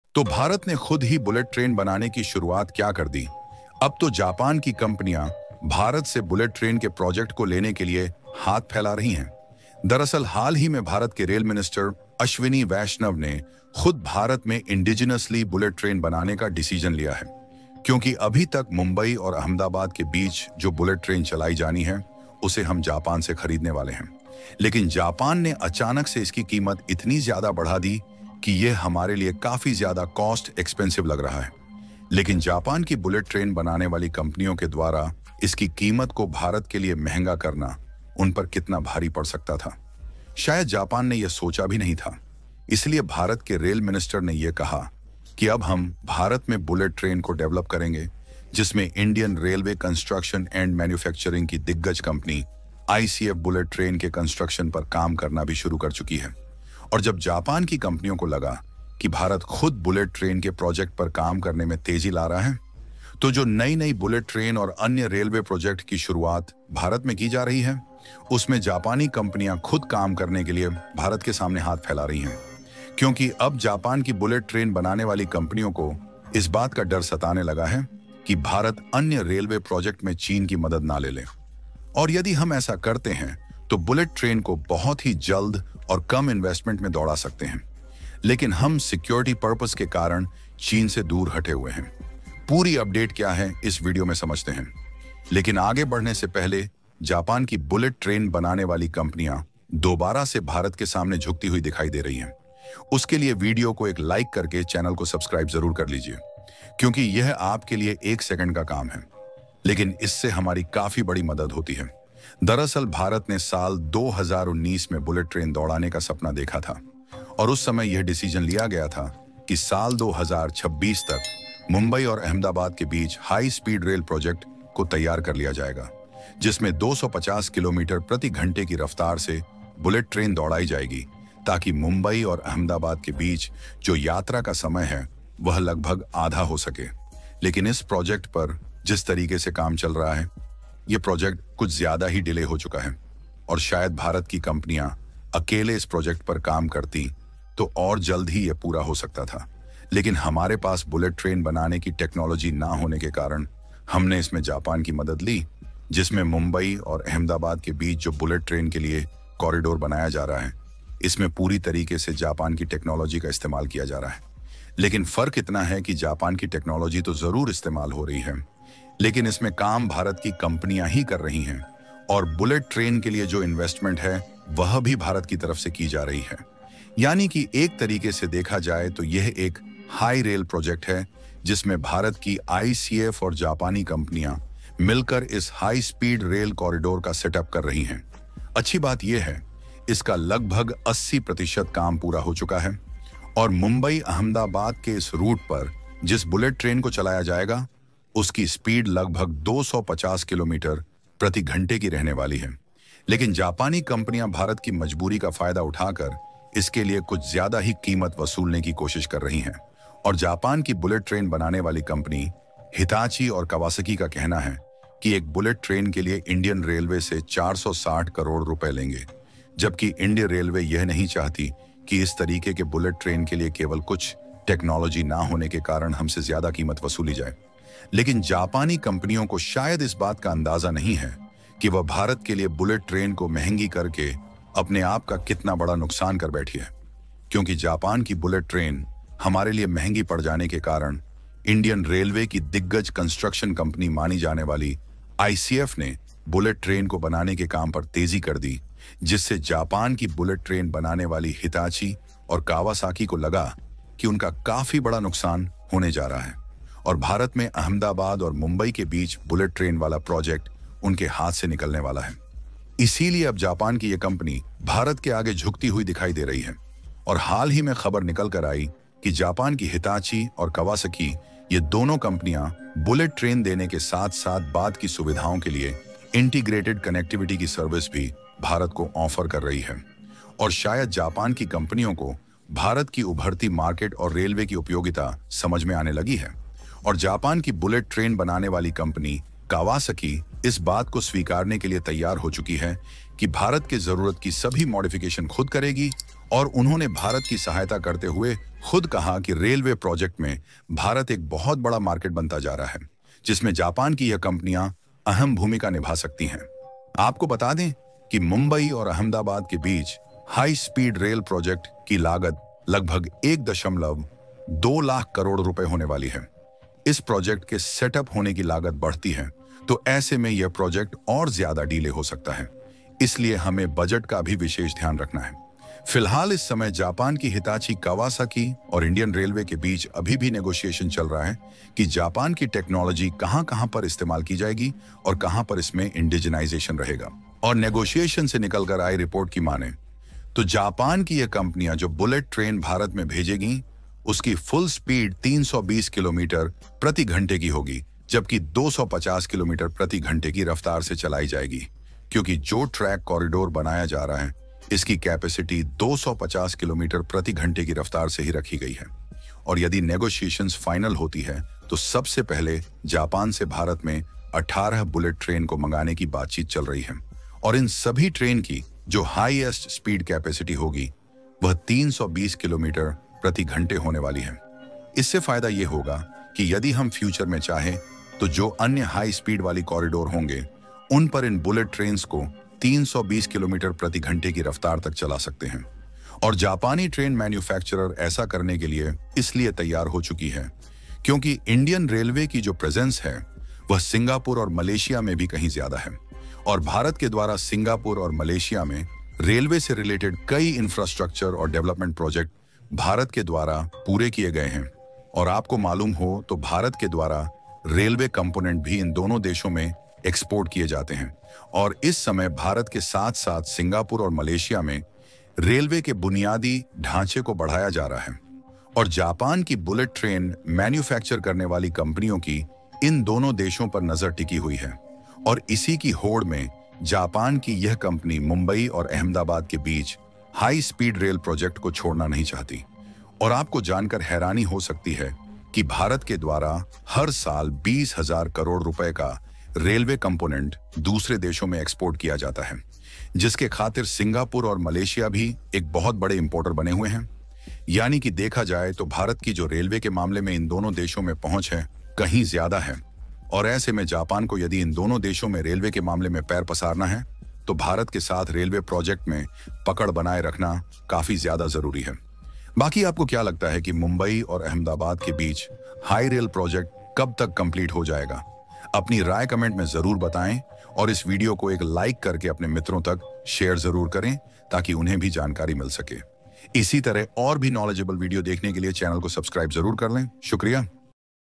आखिर जापान को झुकना ही पड़ा | India Started to Make Bullet Train after Rail Demand
There would soon come a time when high-speed rail (HSR) or bullet train projects might become fully indigenous, Union Railway Minister Ashwini Vaishnaw on Friday said after overseeing a triggered blast in Vikhroli here as part of the Mumbai-Ahmedabad bullet train project which is expected to be completed by 2028.